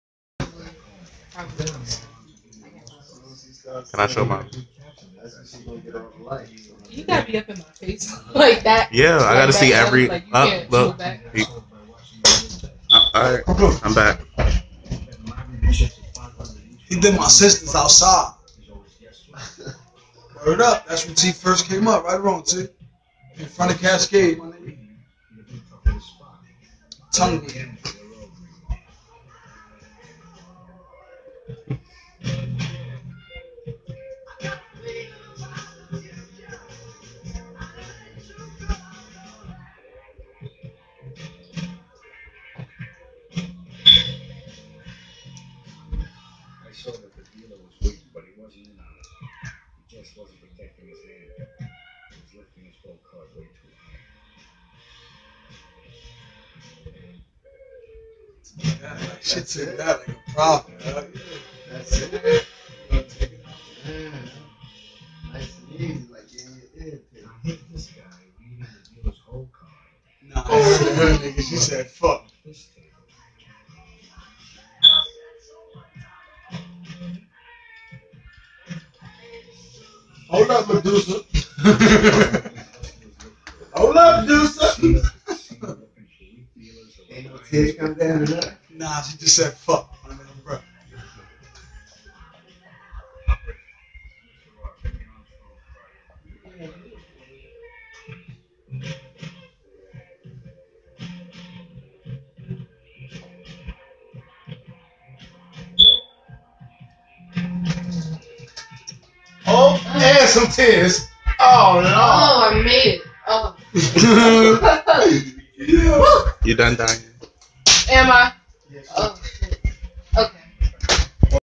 Getting my Medusa/ Philtrum pierced
I was so nervous while having my lip pierced, but I'm happy I got it done.